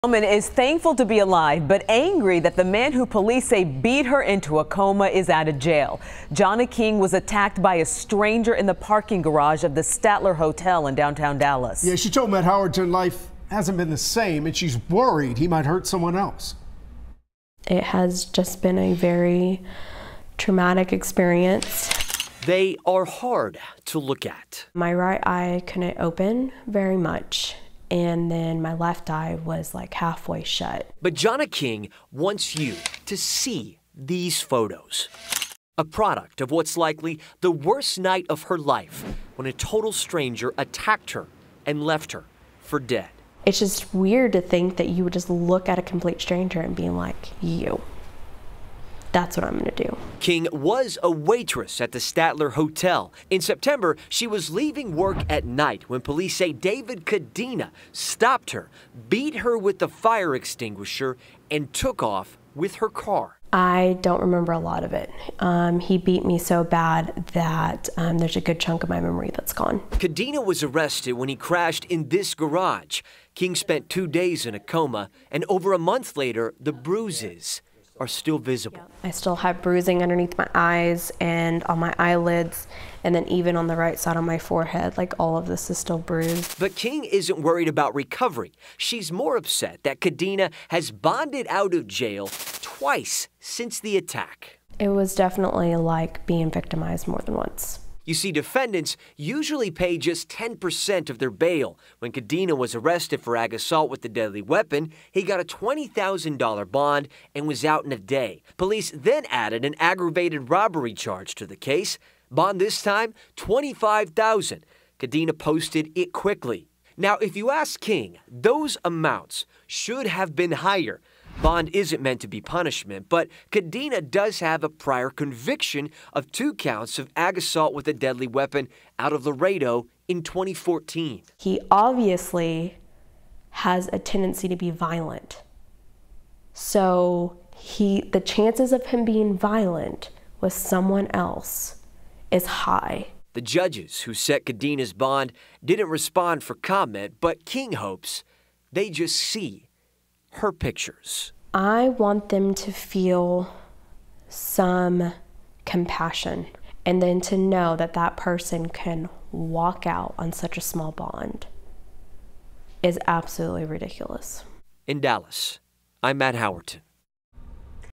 Statler Hotel beating victim: Why me?
Jonna King, 27, was beaten with a fire extinguisher and carjacked by a total stranger following a shift at the Dallas hotel.